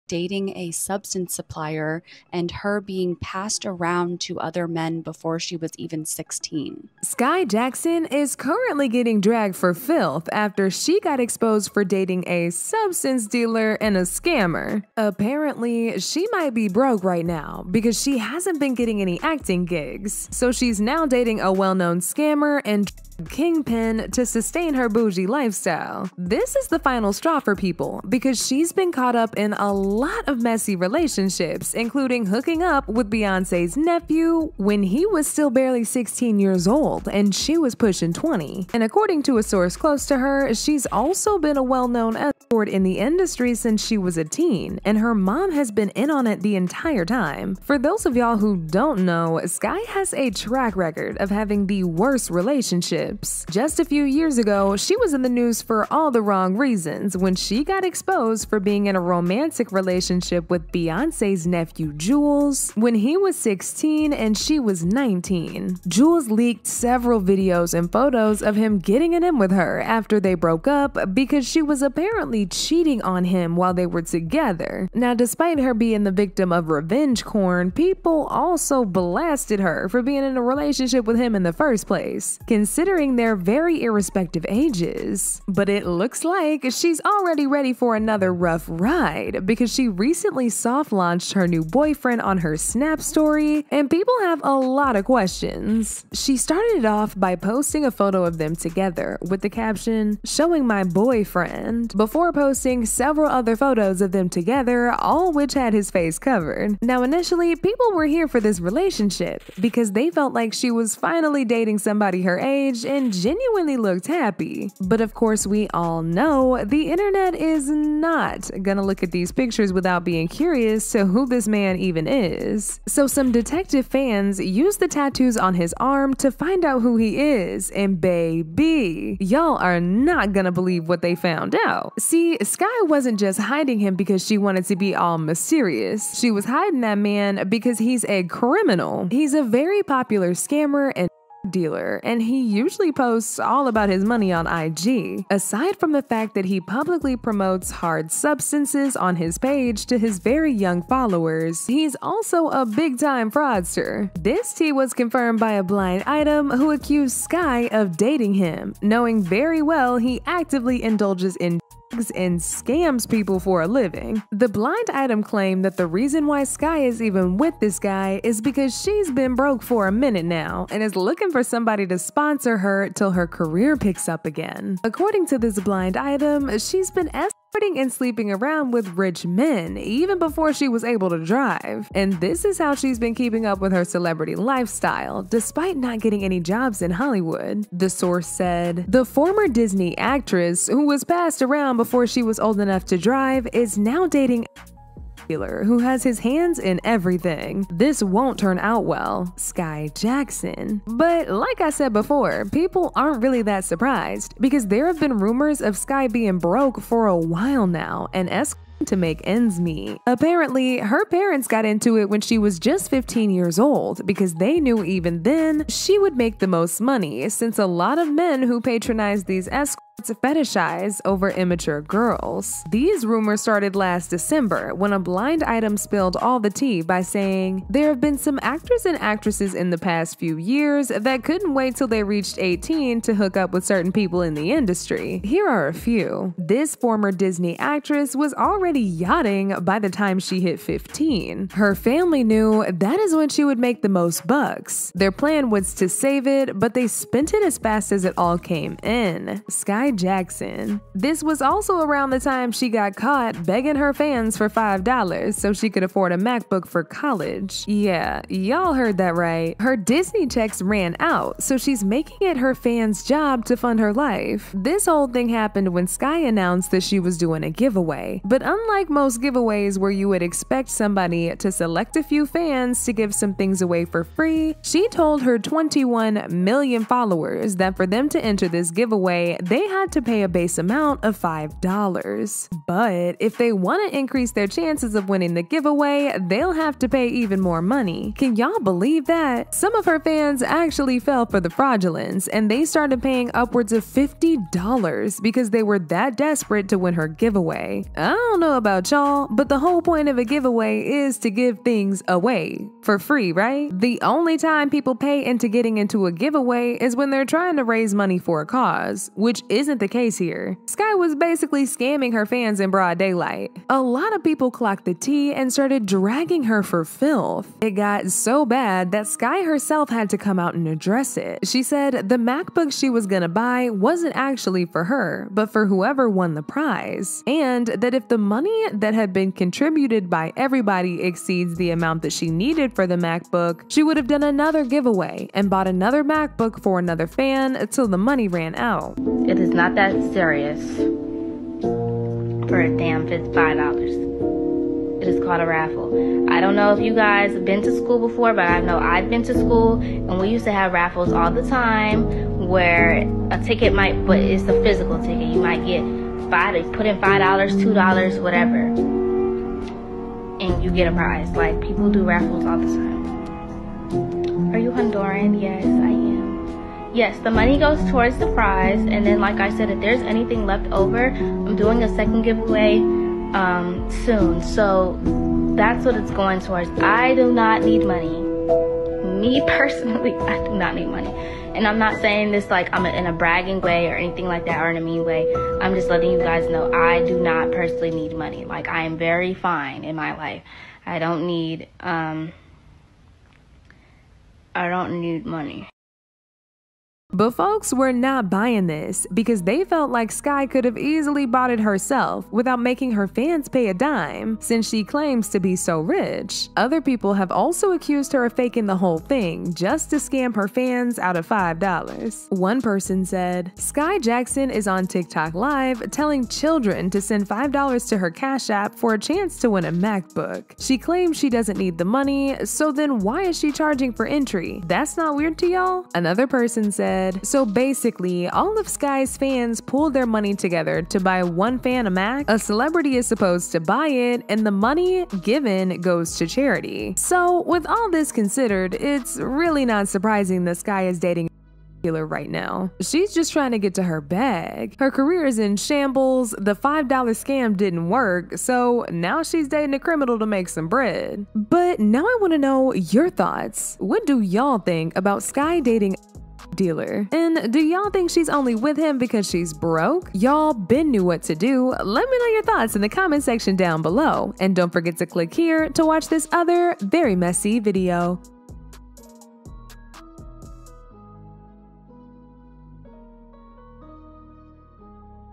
Skai Jackson GOES OFF After Fans Expose Her D3ug Lord Boyfriend
Skai Jackson is currently getting dragged for filth after she got exposed for dating a substance dealer and a scammer. Apparently, she is severely broke right now because she has not been getting any acting gigs, so she is now dating a well known scammer and drug kingpin to sustain her bougie lifestyle.

This is the final straw for a lot of people because she has been caught up in a lot of other messy relationships including hooking up with Beyoncé’s nephew when he was still barely 16 years old and she was pushing 20. And according to a source close to her, she has also been a well known escort in the industry since she was a teenager and her mom has been behind it the entire time.

Despite her being the victim of revenge corn, people also blasted her for being in a relationship with him in the first place considering their irrespective ages.
But it looks like she is already ready for another rough ride because she recently soft launched her new boyfriend on her snap stories and people had a lot of questions.